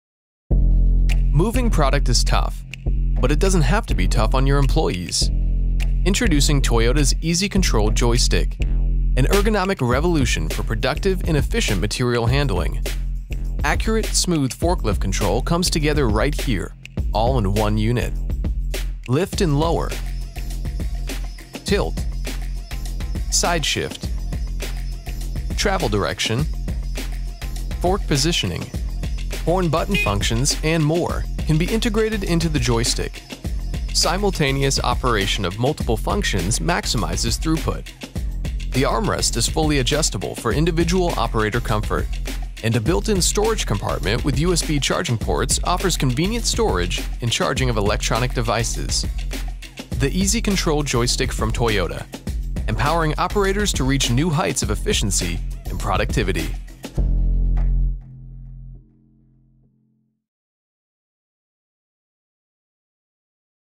Toyota Material Handling Presents: EZ Control Joystick®
Toyota’s EZ Control Joystick® is an ergonomic revolution for productive and efficient material handling, offering accurate and smooth forklift control in a single unit. Toyota's innovative EZ Control Joystick® empowers operators to reach new heights of efficiency and productivity.